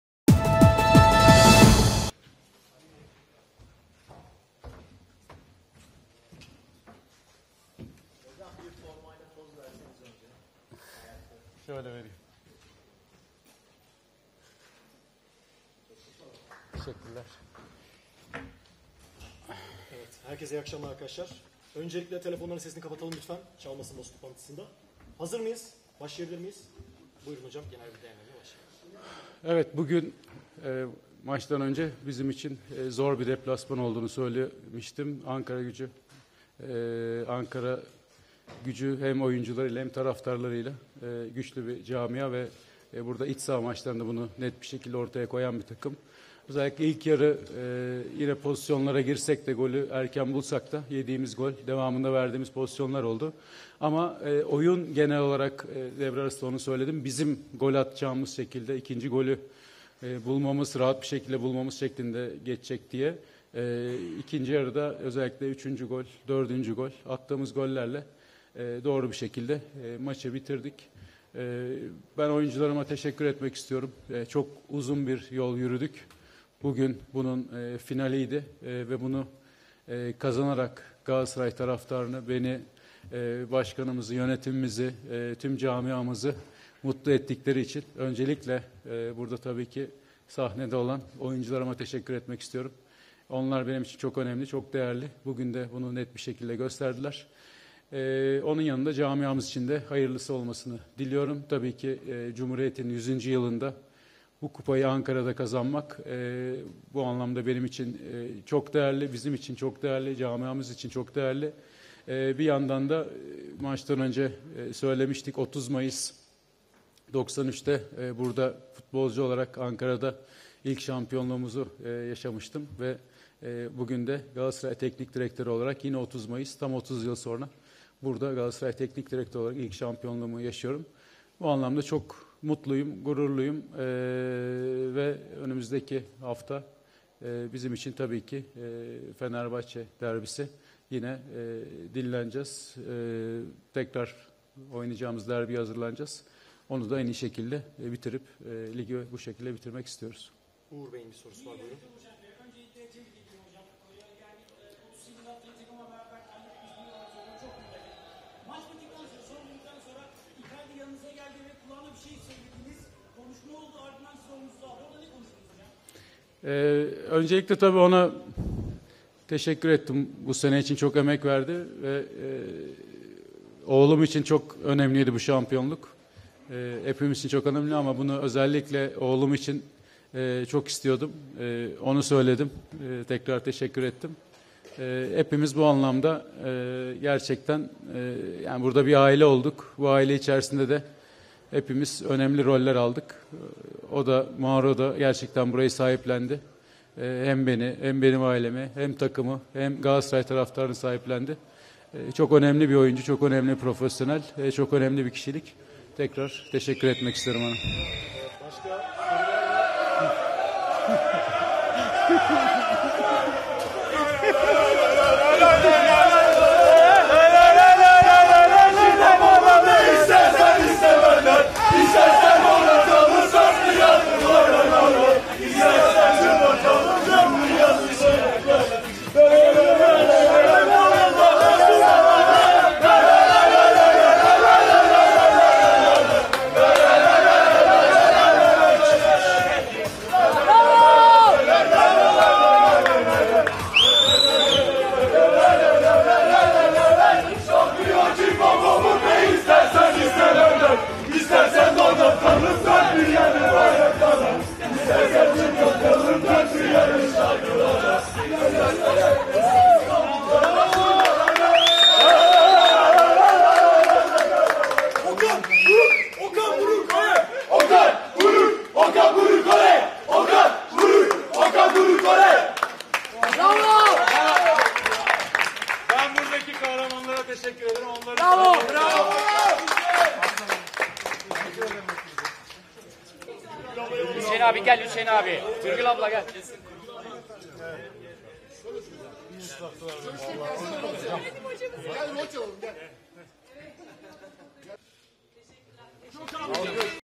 MKE Ankaragücü, Galatasaray maçının ardından Okan Buruk
Spor Toto Süper Lig'in 36. haftasında konuk olduğu MKE Ankaragücü'nü 4-1 yenen Galatasaray, sezonu şampiyon tamamlamayı garantiledi. Karşılaşmanın ardından Galatasaray Teknik Direktörü Okan Buruk basın toplantısında konuştu.